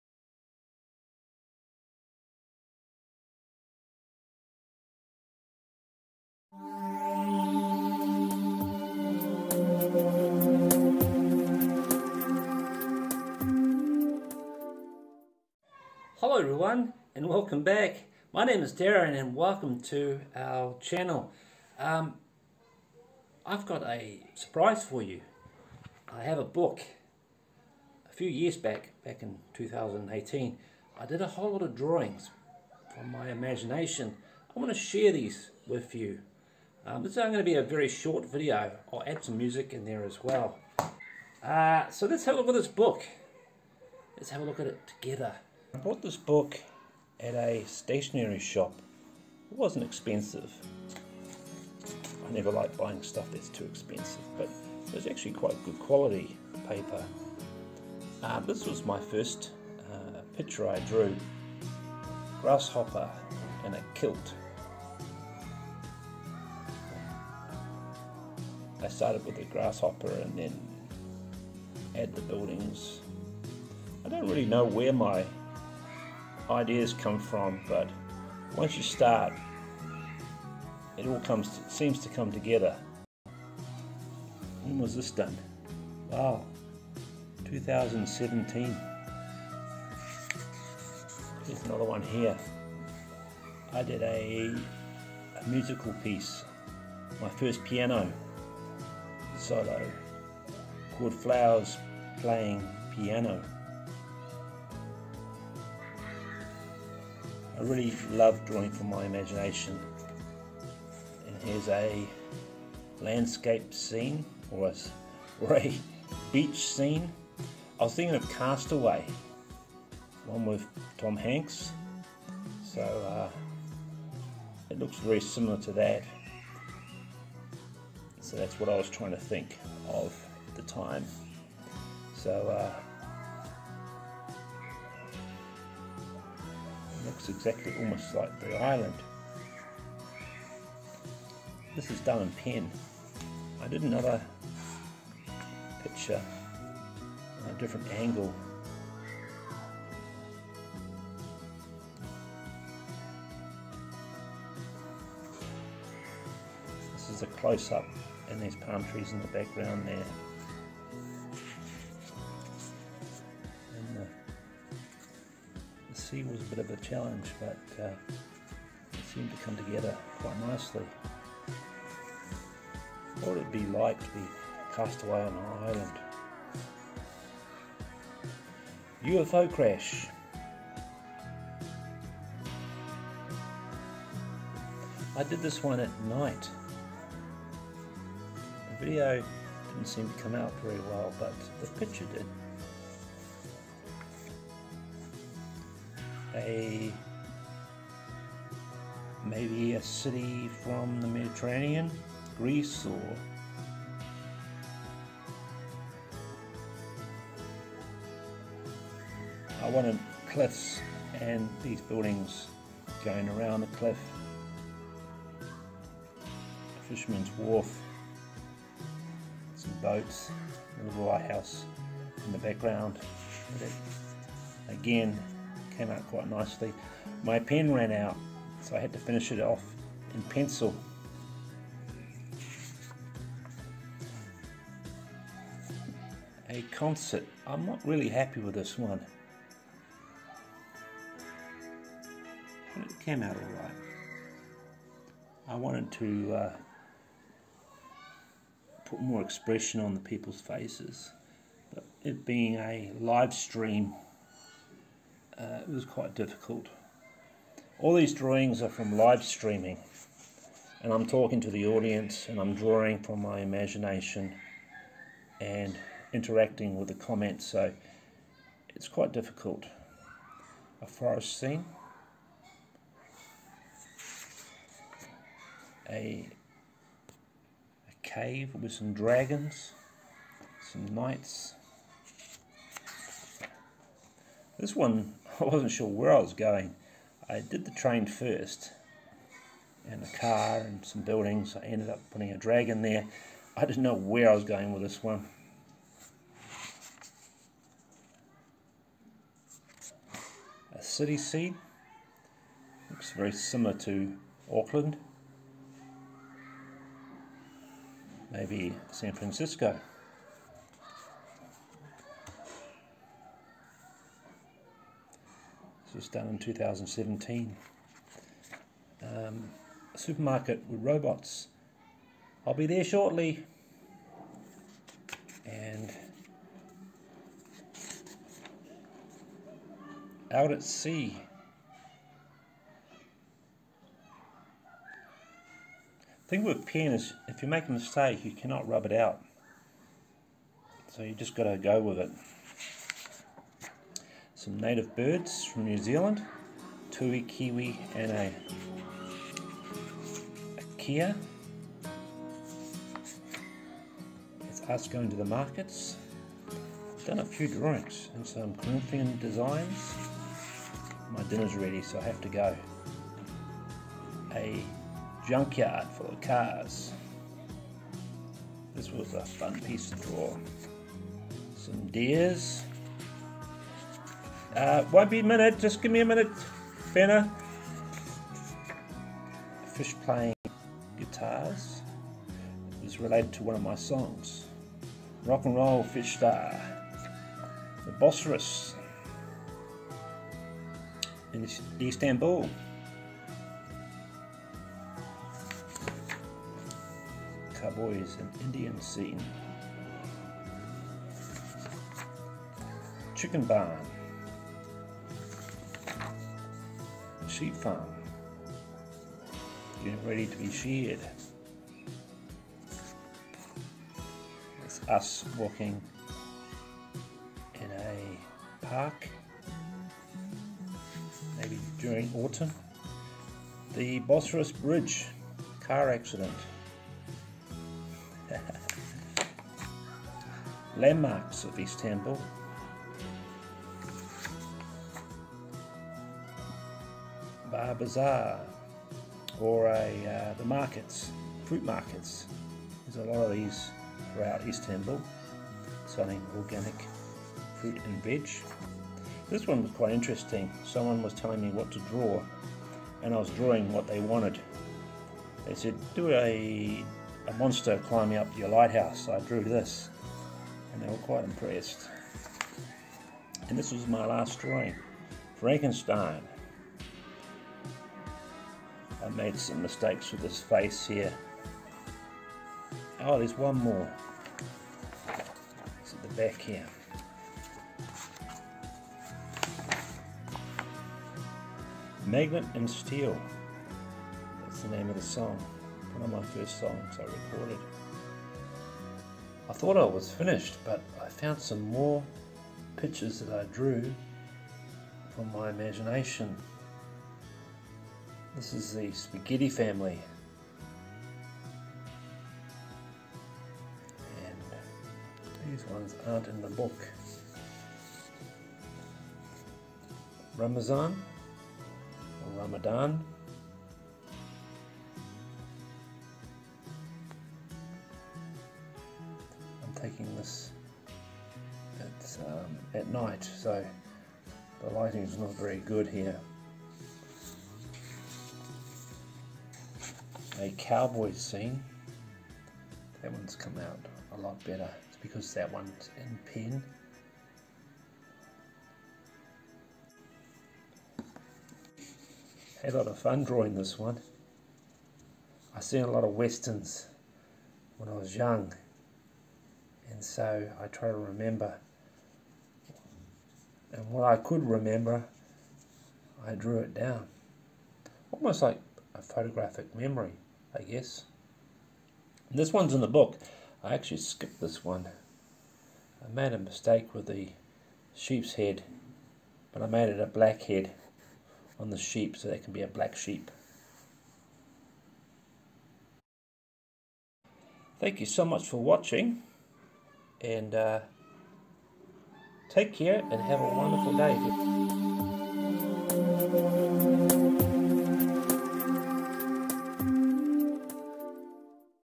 My Imagination Pen Drawings
Join me on a captivating journey through the pages of my 2018 Imagination Sketchbook, where pen strokes bring endless possibilities to life! 🚀✨ Immerse yourself in the boundless realms of my imagination, breaking through outer boundaries with each drawing. Explore the magic of creativity and self expression in this visual adventure.